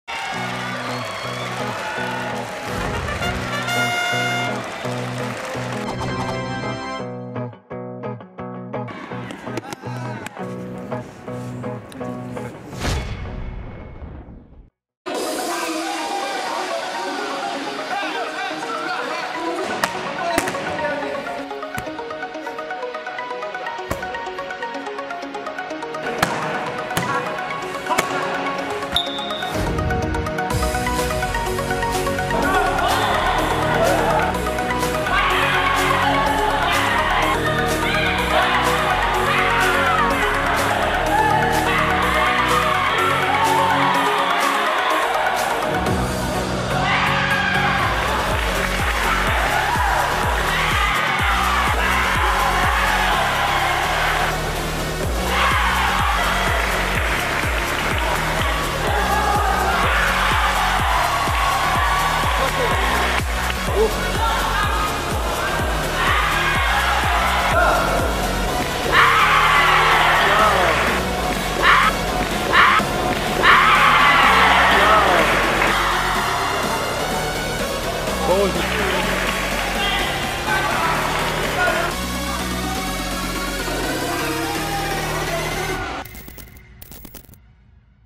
무려 21년간 깨지지 않은 제주의 진기록(feat.고대휴 감독)
서귀중앙교-남주중-남주고-제주대-상무-삼성에스원-제주도청
제77회 전국체육대회 고등부 밴텀급 1위(1996)
세계 군인선수권대회 밴텀급 1위(1998)
제81회 전국체육대회 대학부 밴텀급 1위(2000)
제14회 세계태권도선수권대회 밴텀급 1위(1999)
월드컵 태권도대회 밴텀급 1위(2000, 2002)
제83회 전국체육대회 일반부 밴텀급 1위(2002)